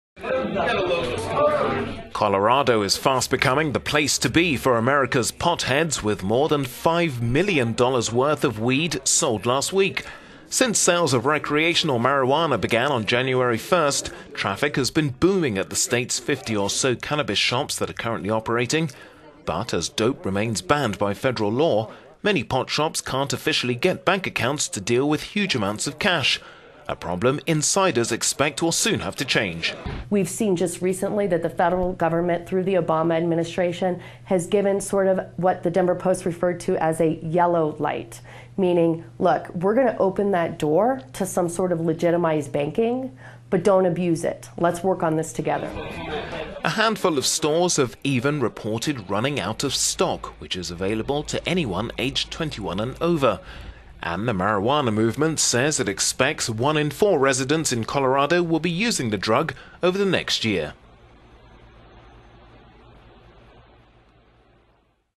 Brisk business for Colorado's booming marijuana trade
Colorado is fast becoming the place to be for America's potheads, with more than five million...

Colorado is fast becoming the place to be for America's potheads, with more than five million dollars worth of weed sold last week.

Since sales of recreational marijuana began on January 1, traffic has been booming at the state's 50 or so cannabis shops that are currently operating.

But as dope remains banned by federal law, many pot shops can't officially get bank accounts to deal with huge amounts of cash, a problem insiders expect will soon have to change.

Attorney Jessica Peck, co-founder of the Women's Marijuana Movement said: "We've seen just recently that the federal government, through the Obama administration, has given what the Denver Post referred to as a 'yellow light' - meaning, look, we're going to open the door to some sort of legitimized banking, but don't abuse it. Let's work on this together."

A handful of stores have even reported running out of stock, which is available to anyone aged 21 or over.

The Women's Marijuana movement says it expects 1 in 4 residents in Colorado will be using the drug over the next year.

Meanwhile some medical officials say the easy access to the drug in Colorado poses health risks, especially to young people.